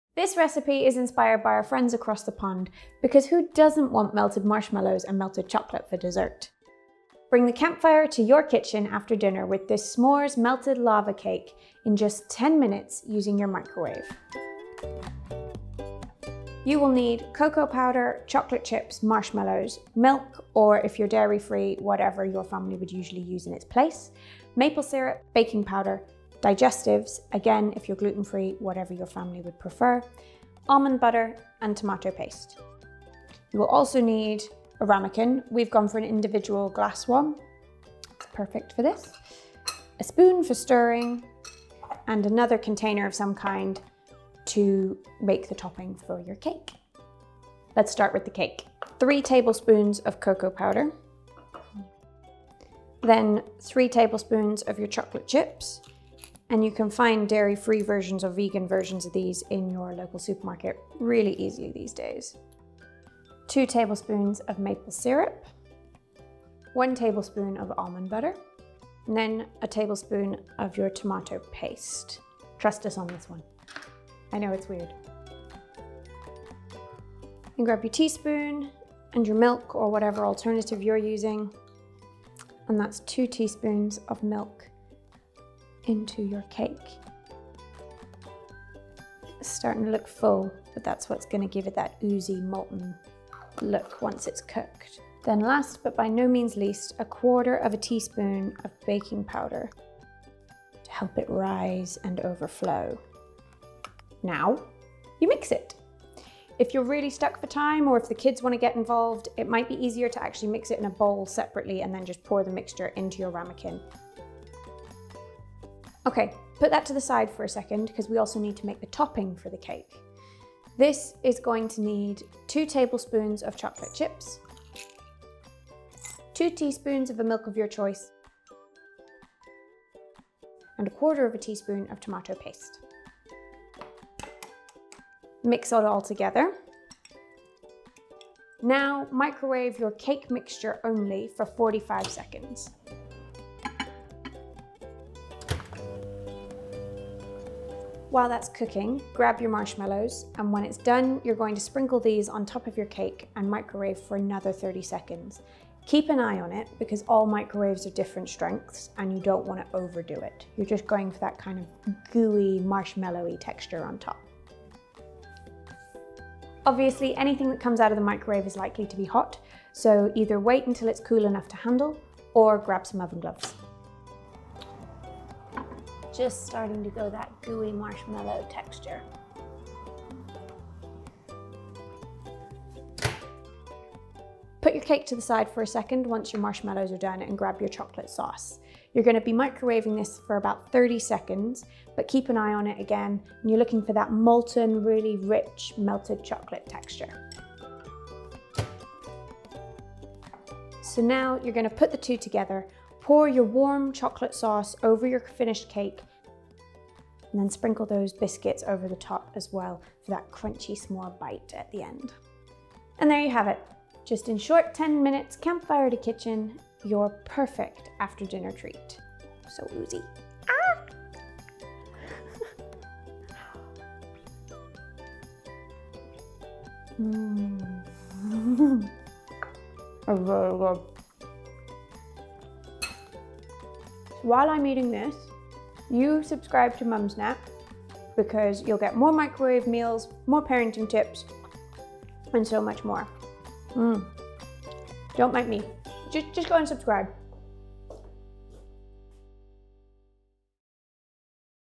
Microwave S'mores Lava Cake
If you're looking for a tasty sweet treat, we have just the thing - a microwave s'mores lava cake! Ready in just 10 minutes, this tasty treat can also be vegan (although during filming we used non-vegan ingredients).

In our latest microwave meal you that great s'mores gooey crunch nice and quick - trust us it's so good!

Ingredients:

Cocoa powder
Chocolate chips
Marshmallows
Milk
Maple syrup
Baking powder
Digestives
Tomato paste

Method:

Combine the following ingredients into a microwavable container for the cake:

3 TBSP of coco powder
3 TBSP of chocolate chips
2 TBSP of maple syrup
1 TBSP of almond butter
1 TBSP of tomato paste
2 TSP of milk
1/4 TSP of baking powder

Mix together and put to the side.

Now mix the following ingredients in a separate microwaveable container to make the chocolate sauce:

2 TBSP of chocolate chips
2 TSP of Milk
1/4 TSP of tomato paste

Now microwave your cake mixture by itself for 45 seconds. Then sprinkle marshmallows and microwave for another 30 seconds.

Set aside and then microwave the chocolate sauce for about 30 seconds.

Then drizzle the chocolate sauce on top of the cake and sprinkle the crushed biscuits on top.

Follow Mumsnet on Instagram: @mumsnet